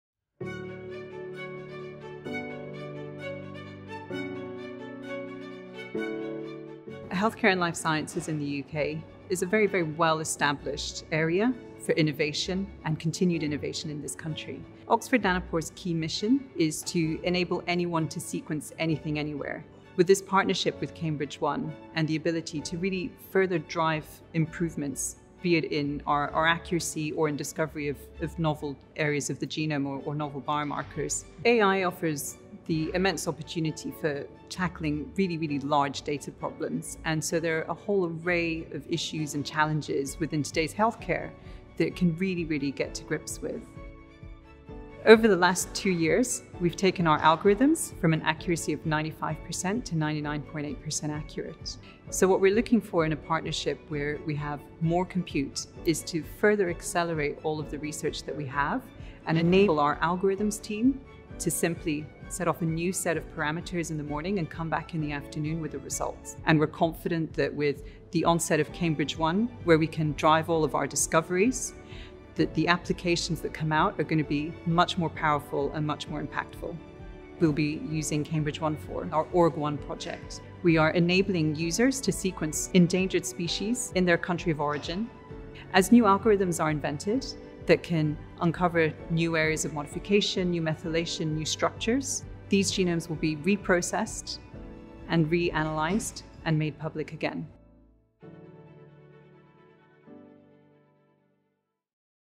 Oxford Nanopore: Driving Discoveries with Accelerated Genomics Research on Cambridge-1
Oxford Nanopore is further accelerating its research with Cambridge-1, enabling its algorithms team to achieve same day results that accelerate research with more powerful and impactful genomics applications.